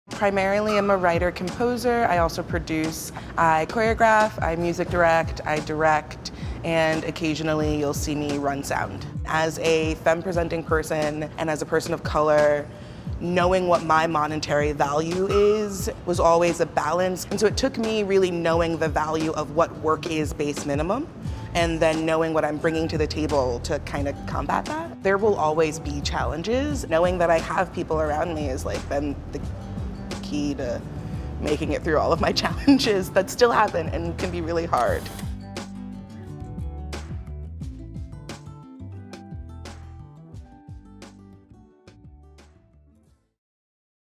Faces of Freelance: Meet Janelle
Faces of Freelance is about shining a light on diverse, talented freelancers and highlighting their successes and experiences in freelancing.